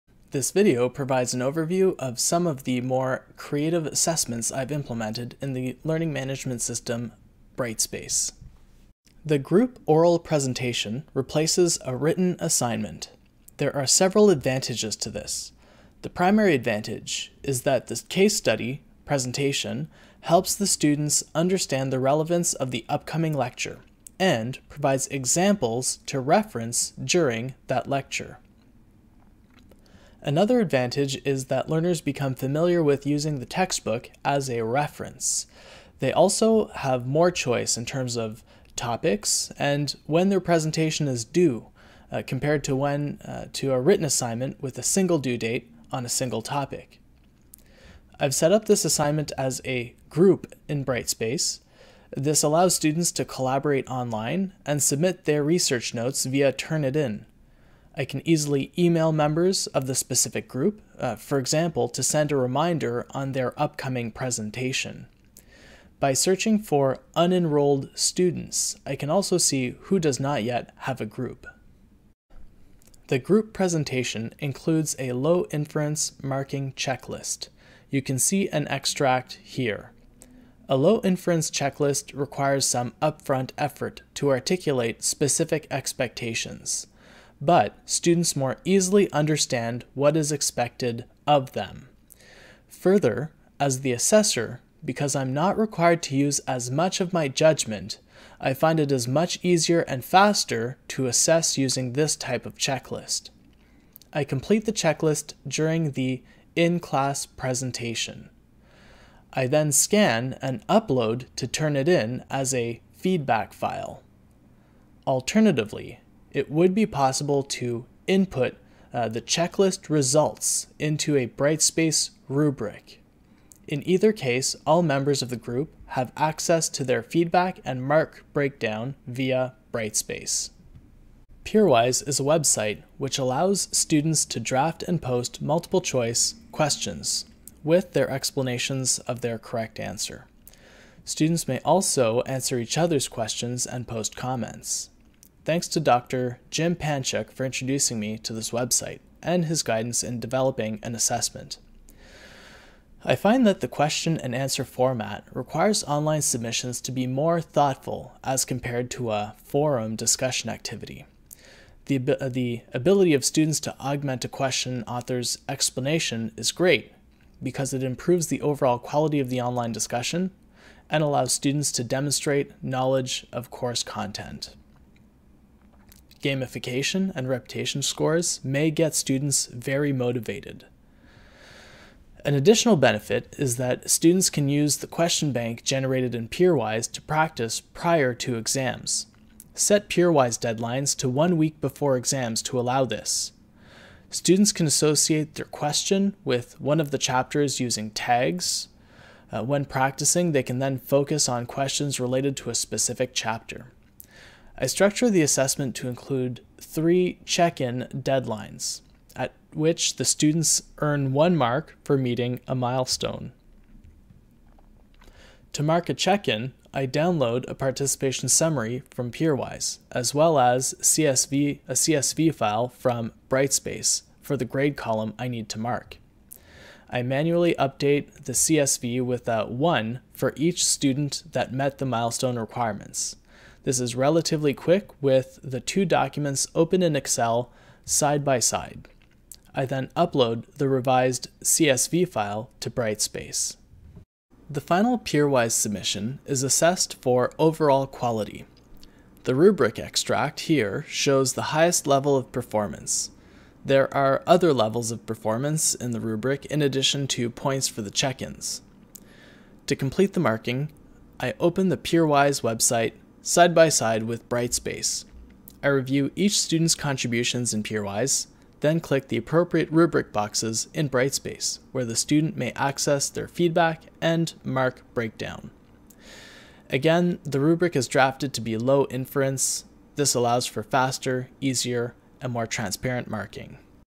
Assessments in Brightspace
An overview of some of the more creative assessments implemented in the learning management system, Brightspace.